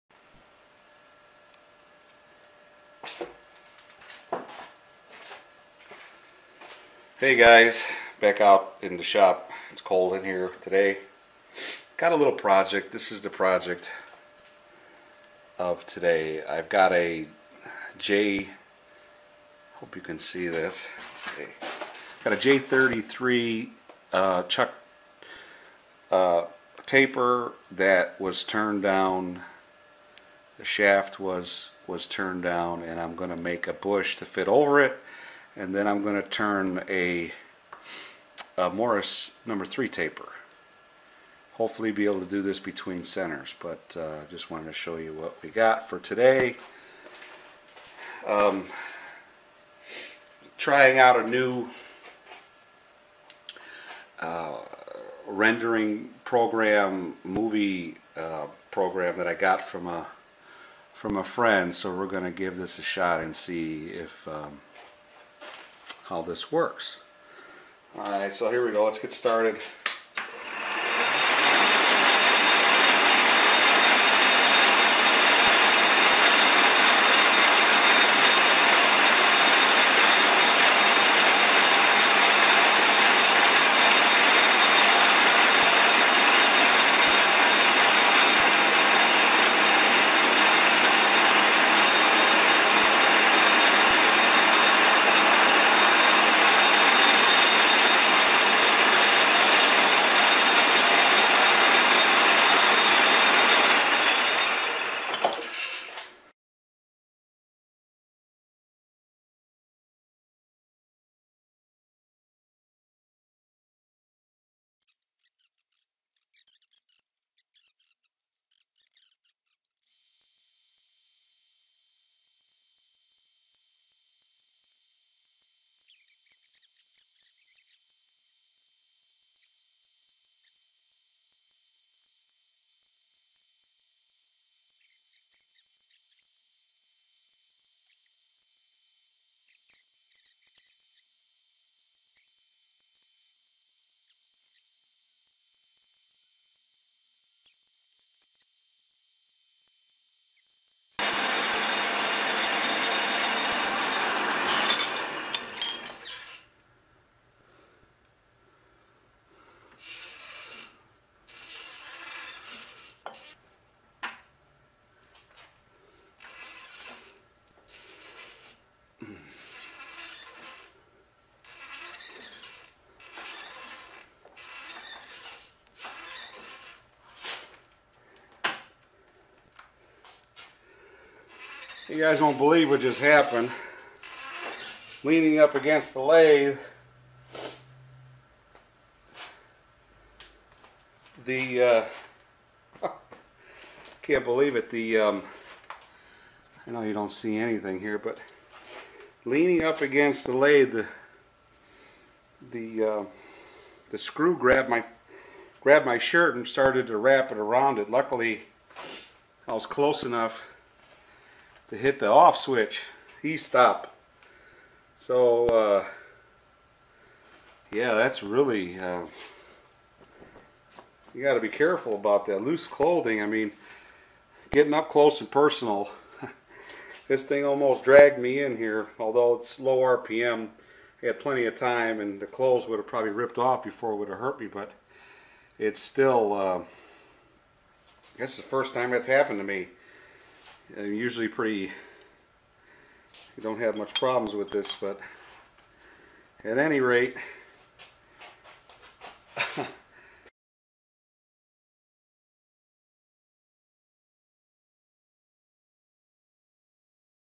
Drill chuck project the making of mt3 taper
had an interesting thing happen while drill out the bush!
new editing software that i am trying out its all experimental!
waiting on a small boring bar..damn it you cannot have enough tools!